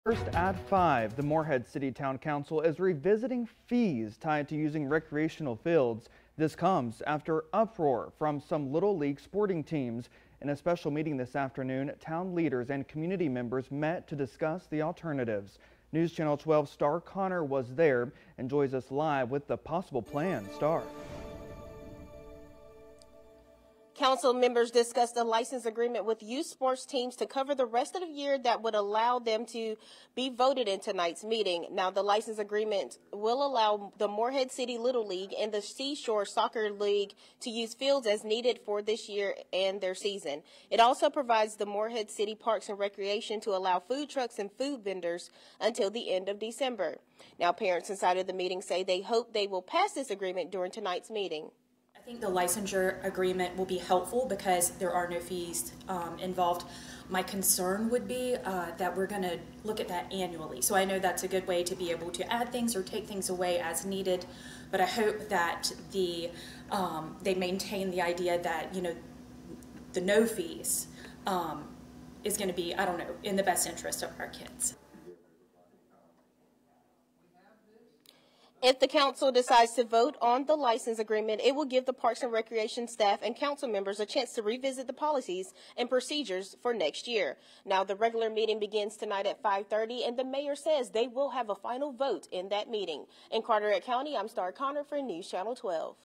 Morehead City leaders vote on recreational field use, parents react
In a special meeting Tuesday, Apr. 11, 2023, town leaders and community members met to discuss alternatives.